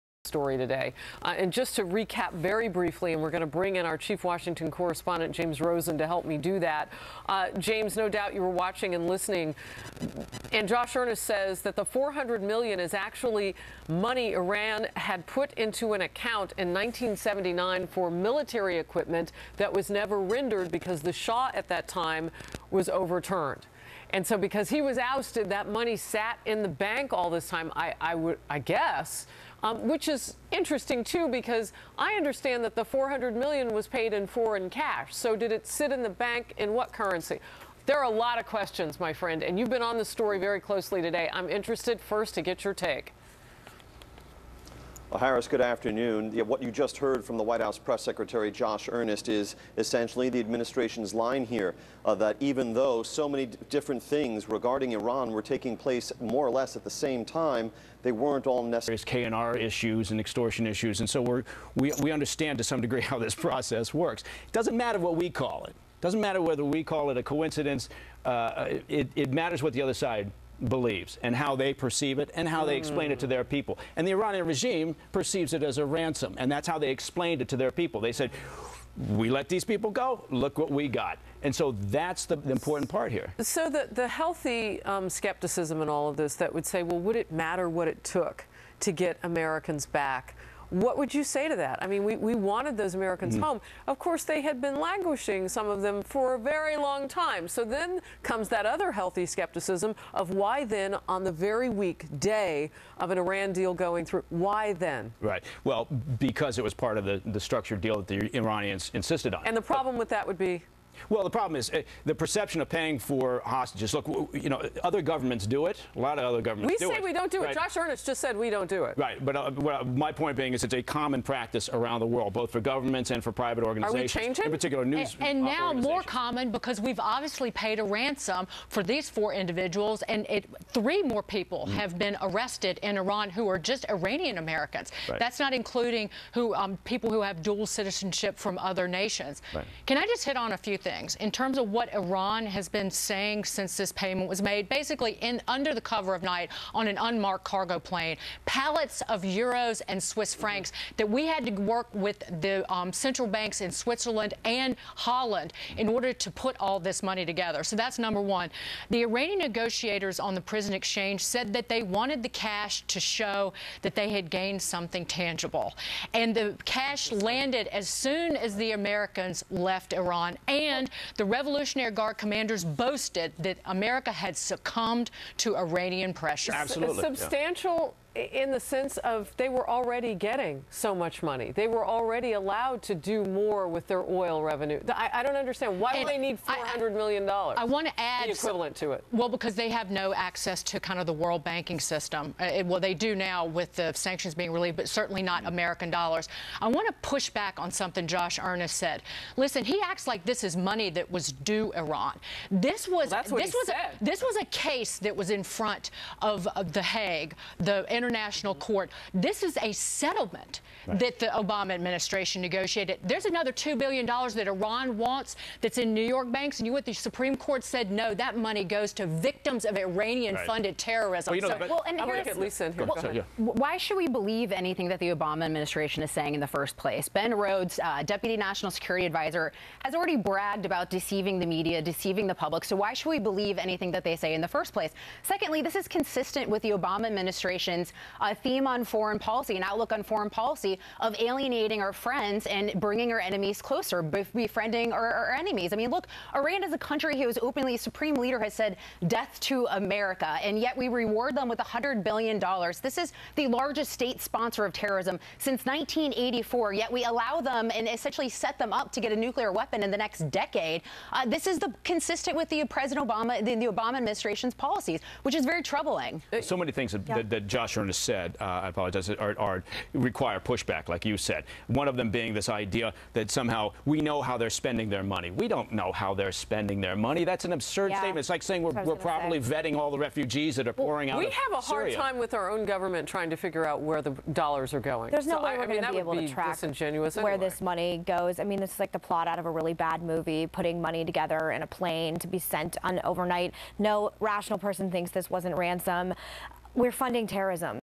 Josh Earnest laughably tries to say we didn't send ransom money to Iran
He wasn't kidding